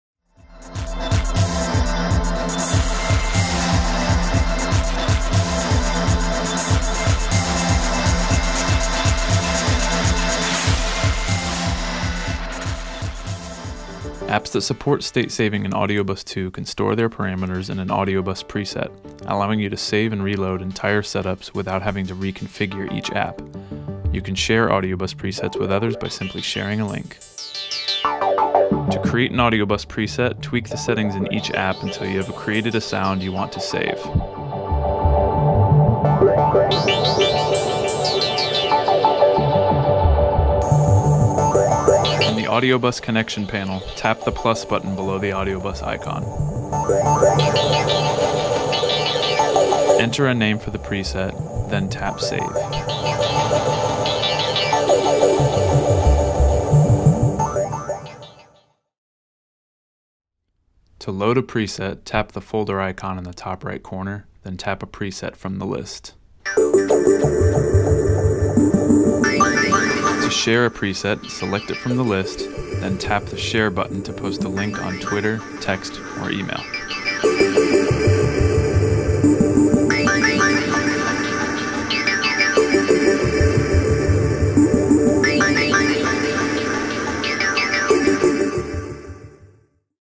Presets in Audiobus 2
In this video Christopher is showing you how to create, load and share presets in Audiobus 2. Don't miss the launch

Some of the apps in the video that are going to support state saving:

And of course Audiobus 2.